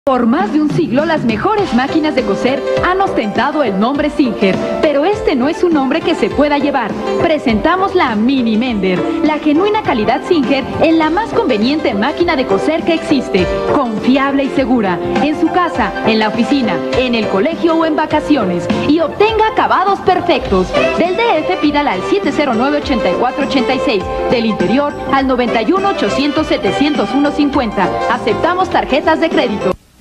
Comerciales mexicanos: Ventel 1992
Producto del extinto ventel a través de Singer cuando las máquinas de coser eran portátiles y ligeras bajo el nombre de Mini Mender